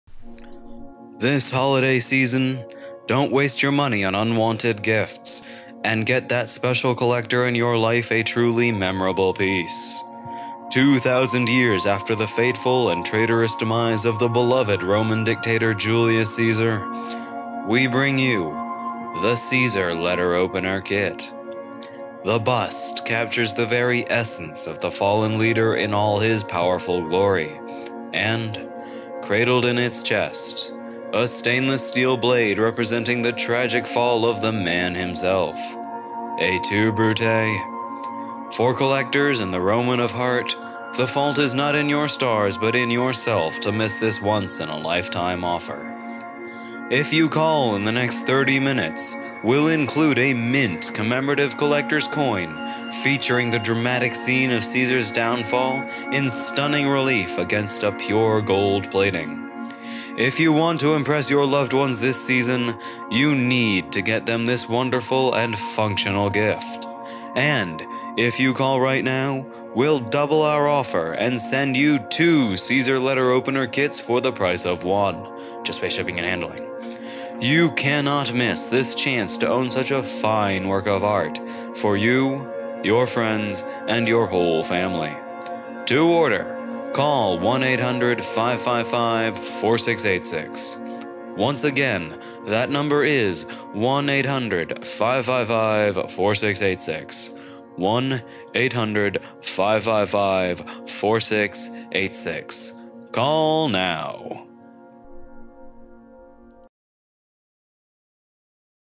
Caesar Letter Opener Commercial
A short comedic video made for an English Class project for the Shakespeare play. Basically it's an artistic effects reel of the bust and blade. The bust was made with MakeHuman and the blade was part of a free VideoCopilot pack for Halloween.